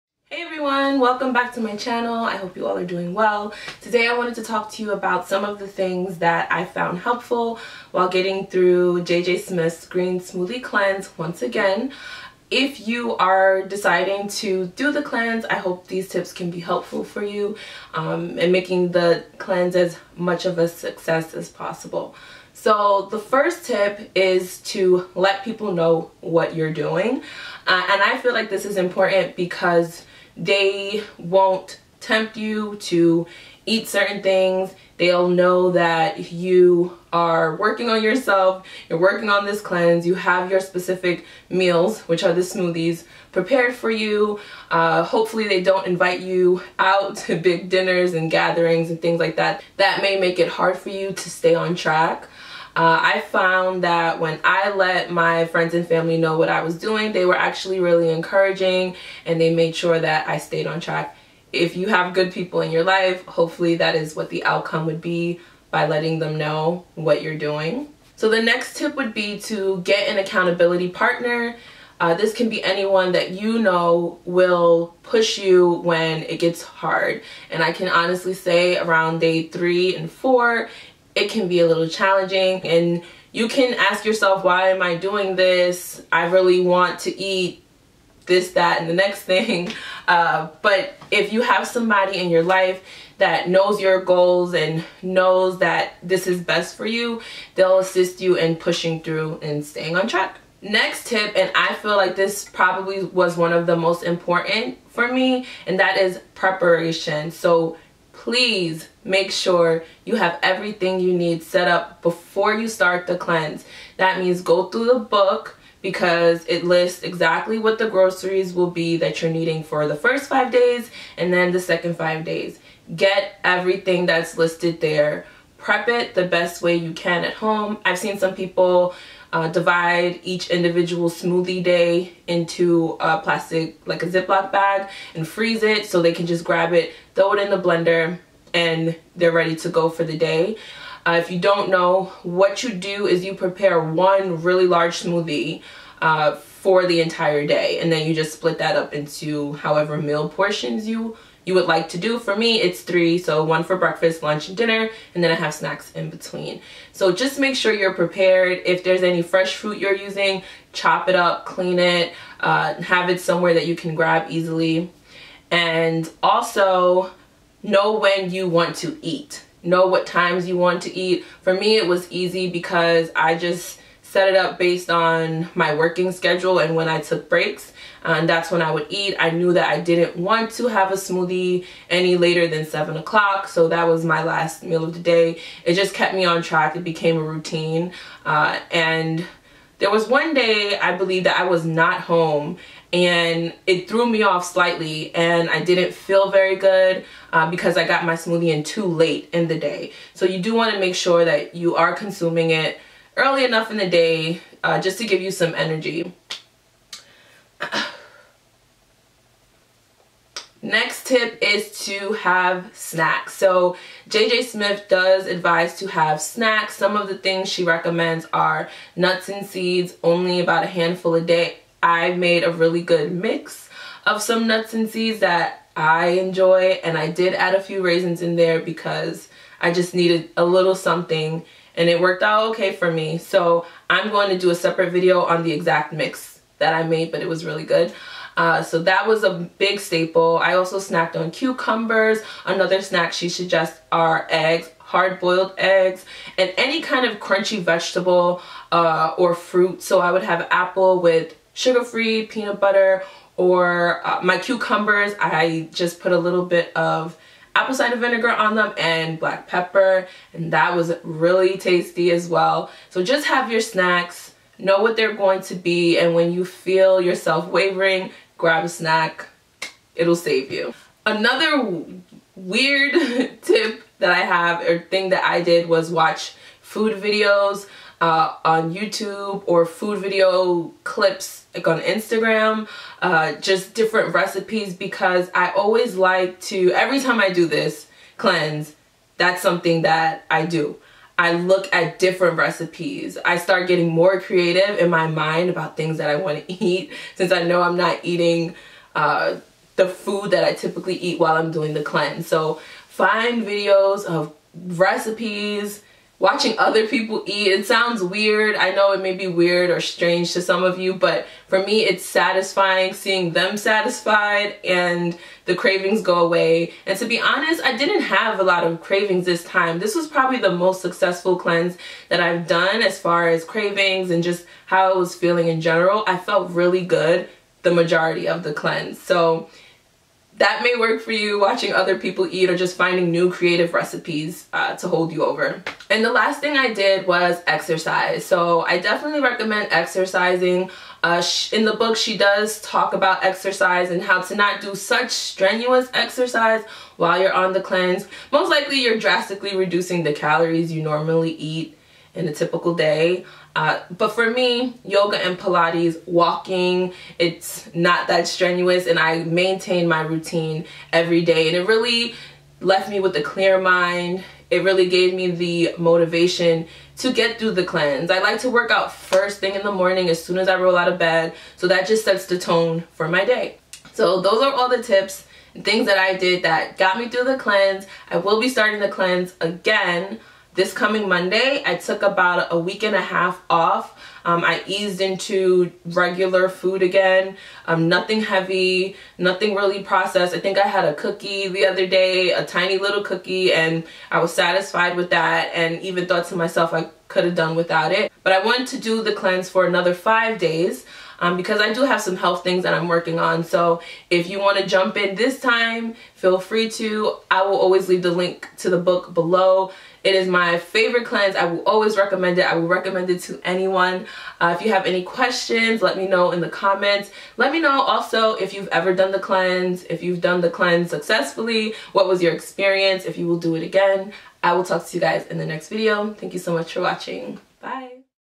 Green Smoothie Cleanse Tips for Success| Sadiya Marie
Sharing some tips that helped me get through JJ Smiths Green Smoothie Cleanse this time around. What are some of your tips during any cleanse or fast?

Link to the Green Smoothie Cleanse Book